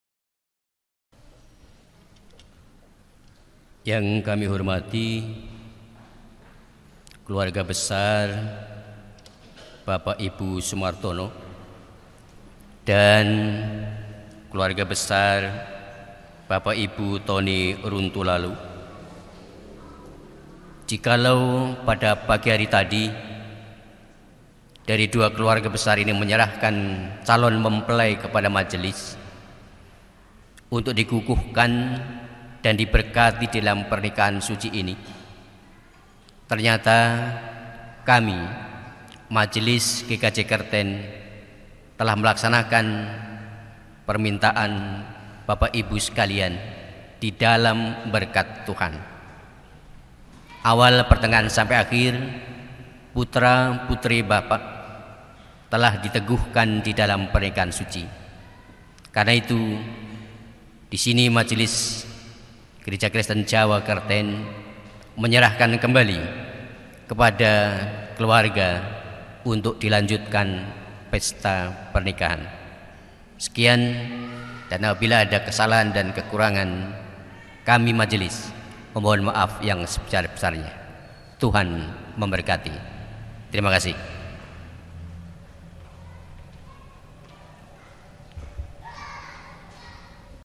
Prakata pembukaan dalam rangka pernikahan Wisnu & Shendy 7 Juli 2012
Pembukaan dalam rangka pernikahan Wisnu & Shendy 7 Juli 2012

Mohon maaf kalau gambar videonya kurang maksimal, karena sudah beberapa kali proses kompres.